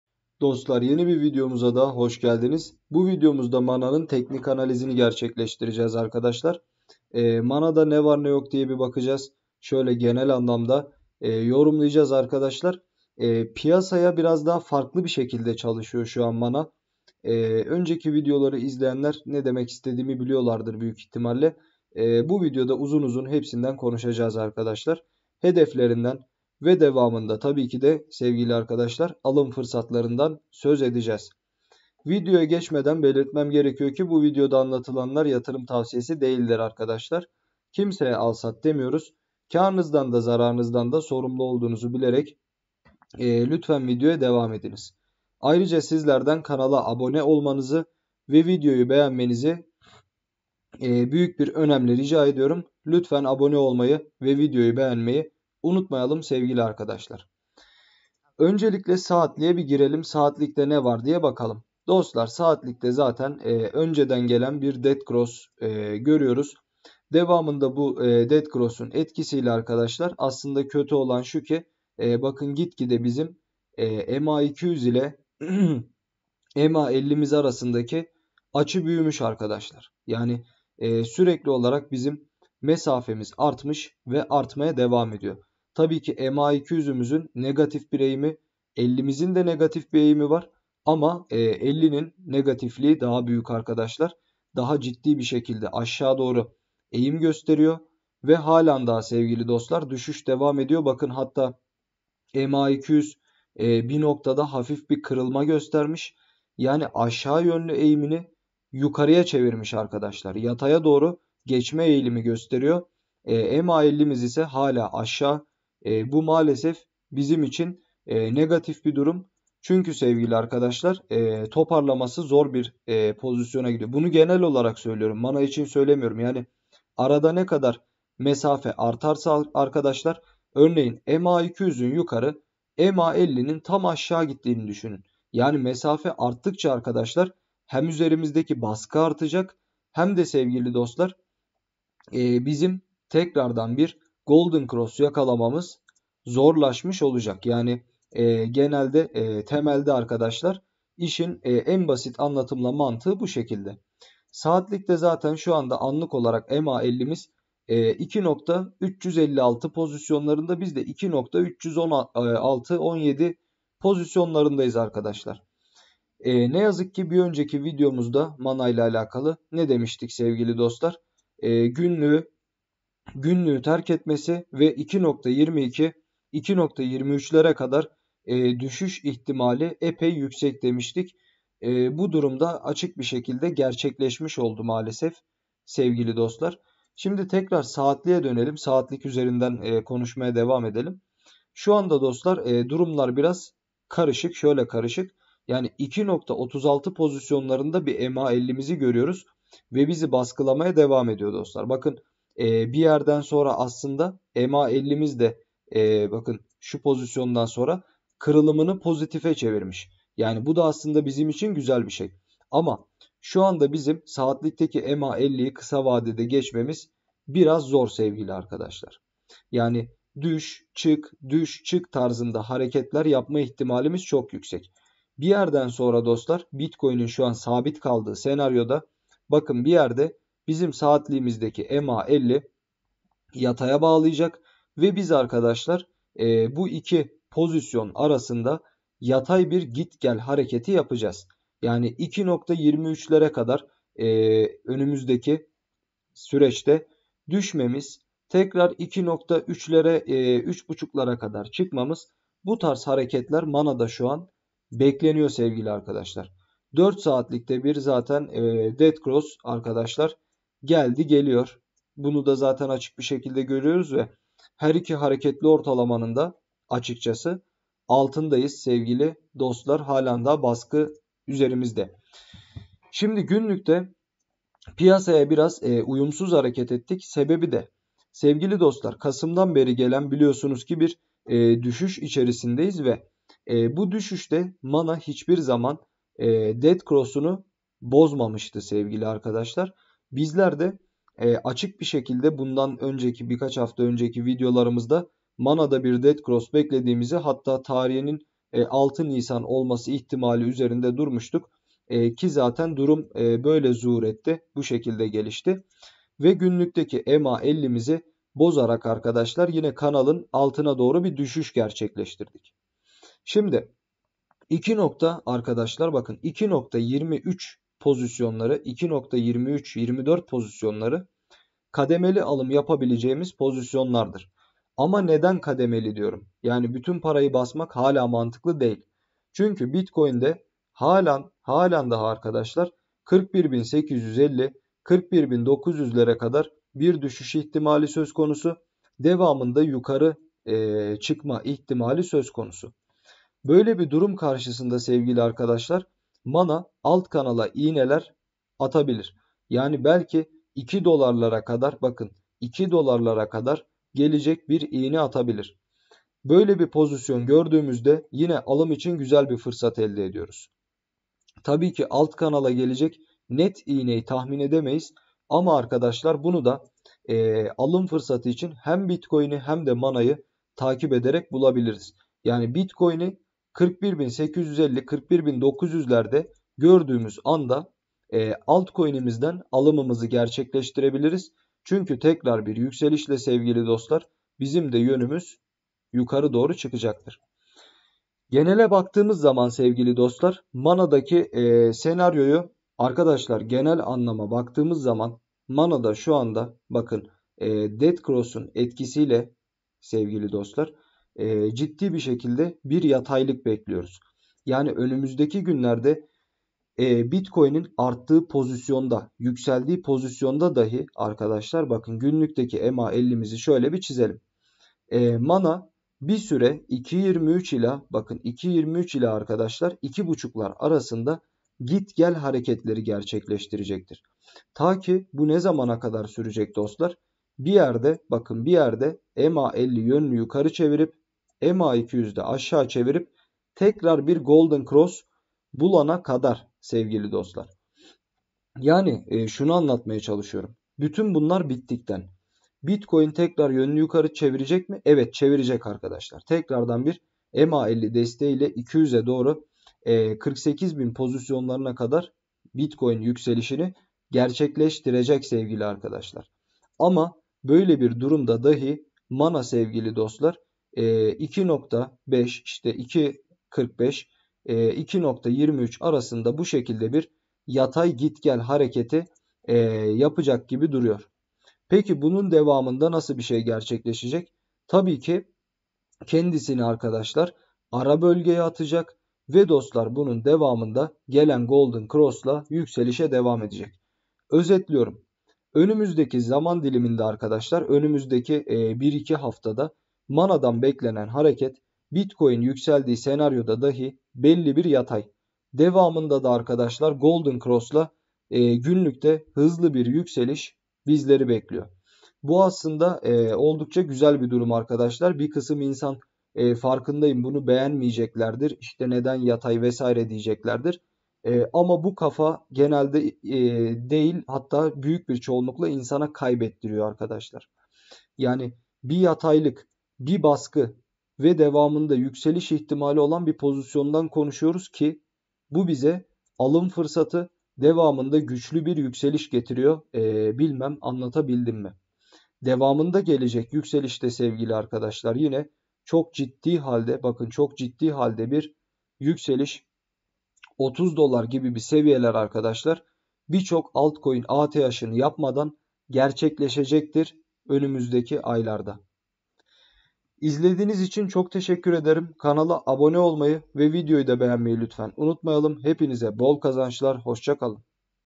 MANA COİN BÜYÜK RALLİ! TEKNİK ANALİZ!🚀HEDEF 30$!🚀PİYASAYI SOLLAYACAK YÜKSELİŞ!🚀SON FIRSATLAR! PUMP!🚀
Bu videomuzda Metaverse projesinin demirbaş alt coinlerinden olan Mana alt coinini değerlendirdik. Kripto borsasının popüler coinlerinden olan Mana'nın kısa, orta ve uzun vade hedeflerini belirttik ve destek direnç noktalarını da belirleyerek alım satım noktalarına da değindik. Mana coin, günlükte MA50 üzerine atarak büyük yükselişin temelini attı ve 3.5$ dolar fiyatlarına kadar kısa vadede yükselme potansiyeli bulunuyor. Bu videoda yatırım tavsiyesi verilmemektedir.